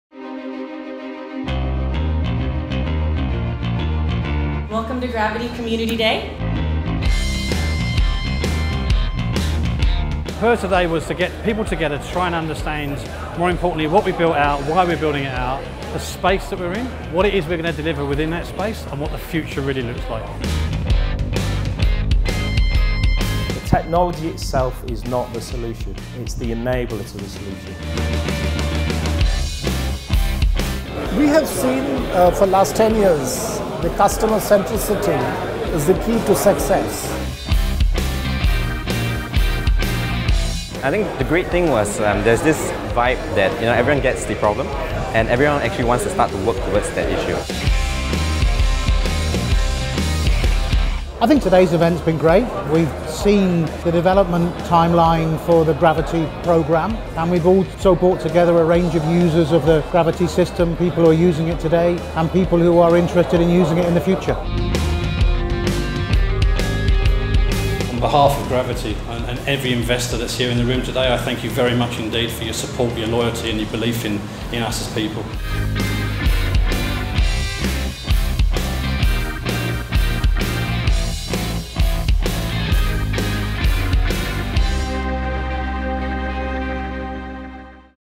Gravity Community Day 2017 - Gravity Supply Chain
Community Day is about bringing together Gravity users, customers, partners, prospects and investors for a day of networking and presentations. It’s an extension of what we deliver to our customers through the Gravity platform – collaboration within the supply chain network and a community for businesses to trade within.

to see photos and learn more about Gravity Community Day.